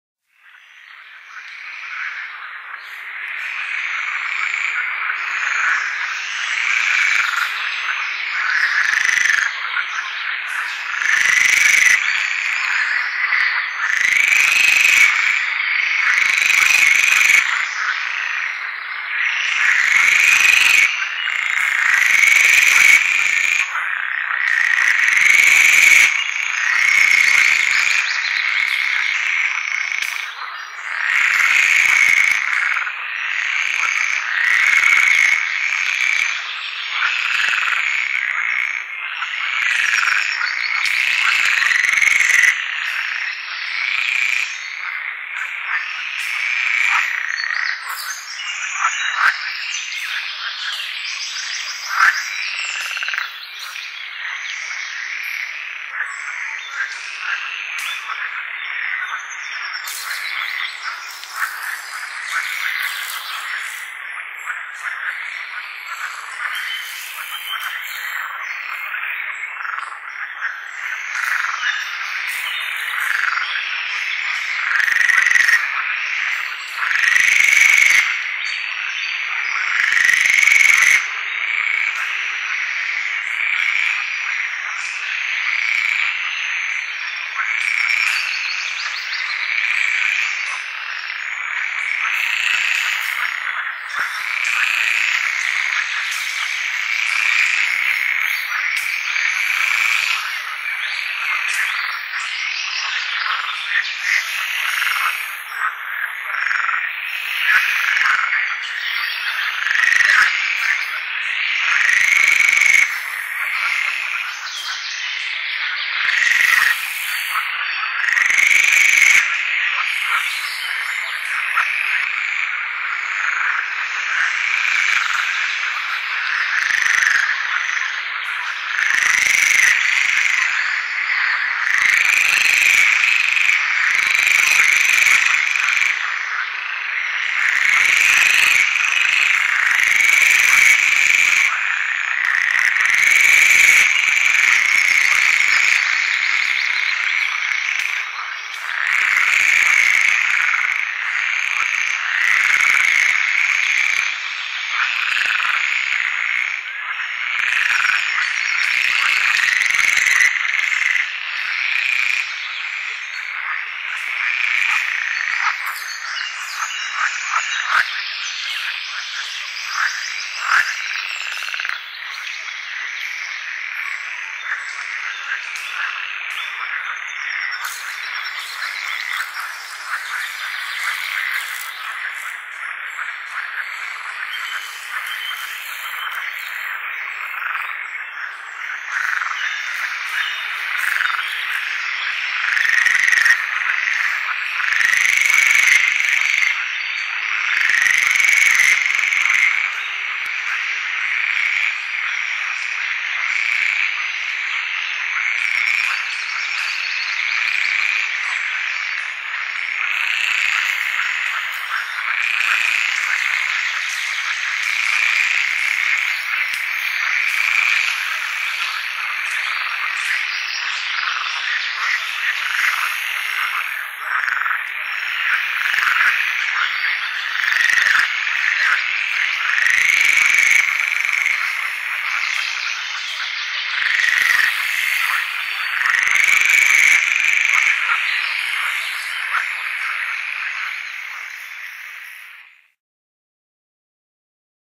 żabi soliści stereo oraz głośny śpiew ptaków zięby, piecuszka, śpiewaka
Głosy leśnych ptaków bez podkładu muzycznego.
Poznaj otaczającą Ciebie przyrodę.
Dzięki odpowiednio opracowanemu albumowi z odgłosami ptaków, możesz poznać bliżej niezwykły świat przyrody i zrelaksować w zaciszu domowym, jak na łonie natury...